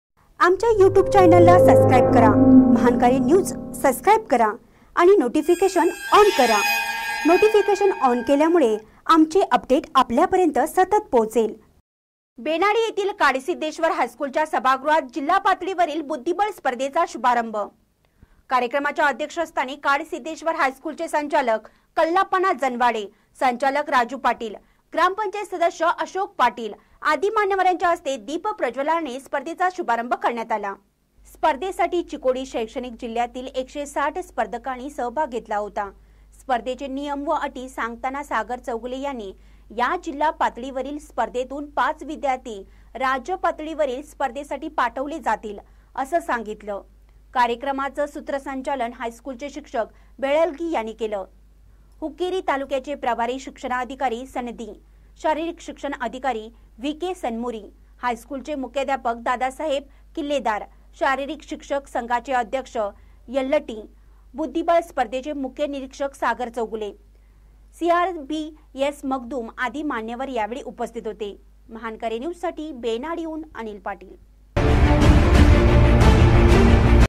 बेनाडी येथील काडसिद्धेश्वर हायस्कूलच्या सभागृहात जिल्हापातळीवरील बुद्धिबळ
बेनाडी येथील काडसिद्धेश्वर हायस्कूलच्या सभागृहात जिल्हापातळीवरील बुद्धिबळ स्पर्धेचा शुभारंभ